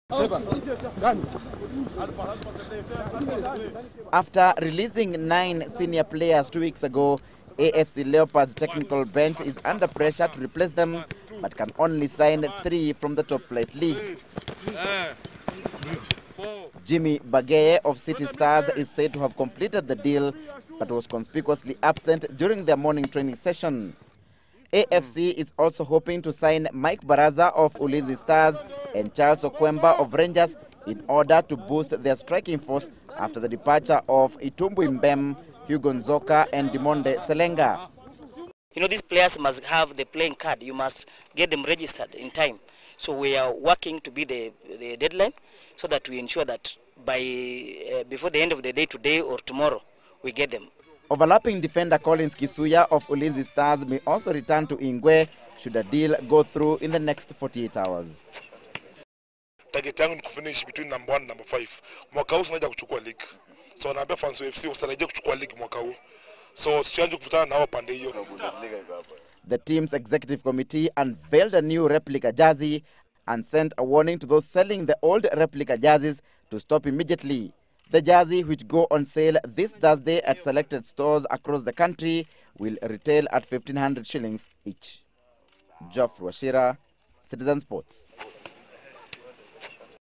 AFC Leopard's New Signings
AFC Leopards are planning to sign 3 top players from other Kenyan Premier League clubs before they face Sofapaka on the 28th of this month. Leopards have approached mike Baraza and Collins Kisuya of Ulinzi stars, old boy Charles Okwemba of rangers, Francis Chinjili of Western Stima, and Jimmy Bageye of city stars, but they can only sign three. Meanwhile the team has unveiled new replica jersey, which will be retailing at 1,500 shillings.